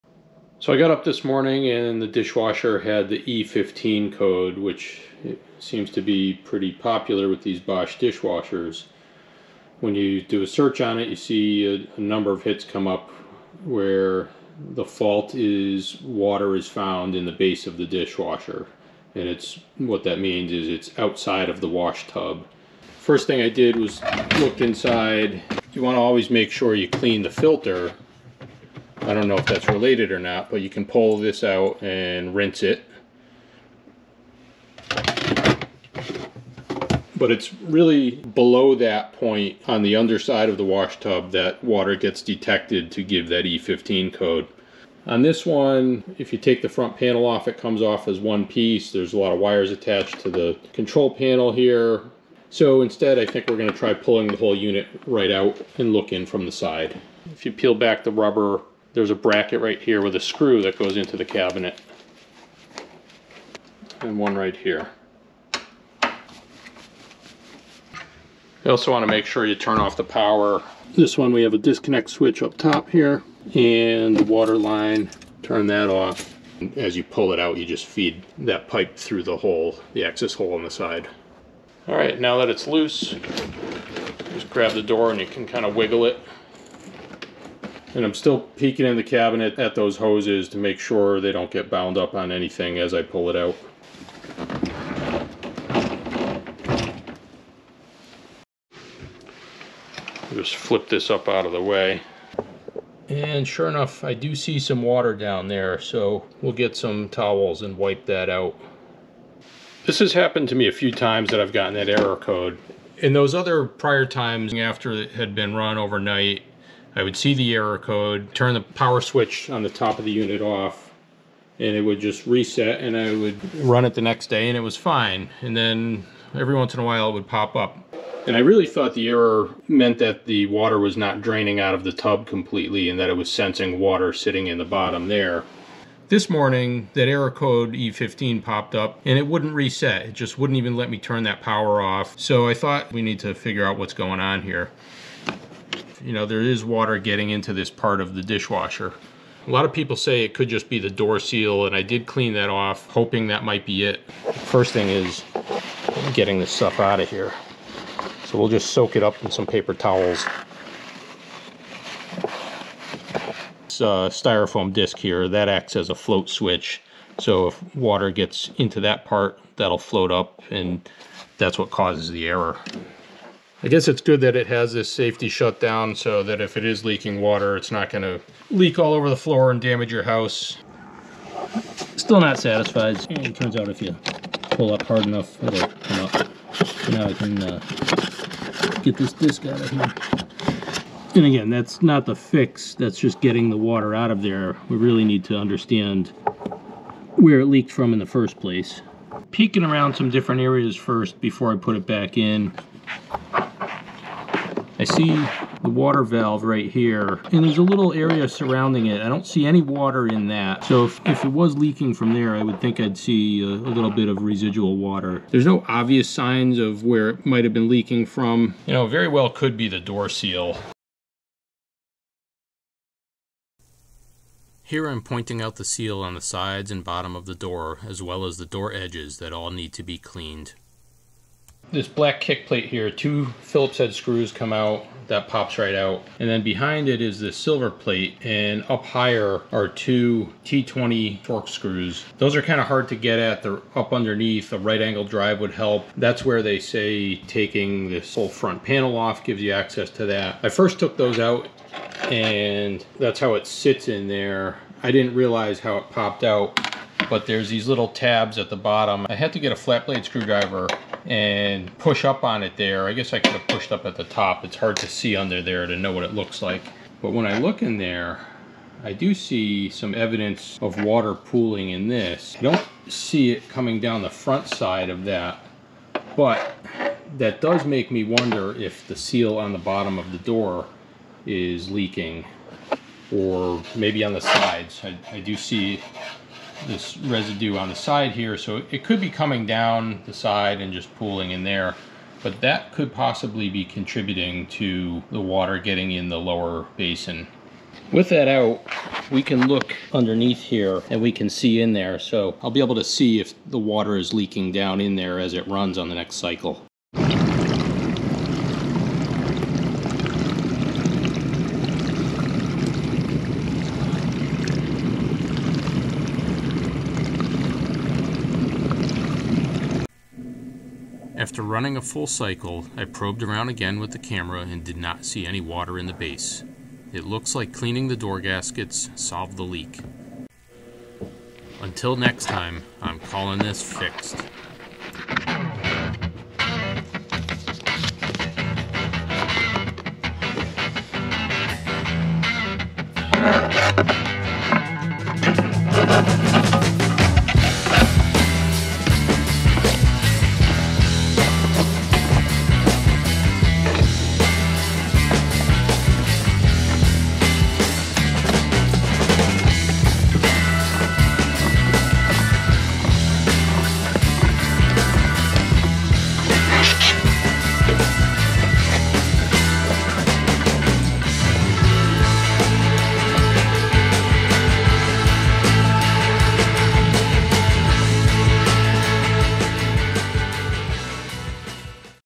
Bosch Dishwasher E15 Code
I find a simple fix to the E15 error code found on my Bosch 800 series dishwasher.  I hope this continues to work.

This right angle drive is very handy for getting at screws in tight places: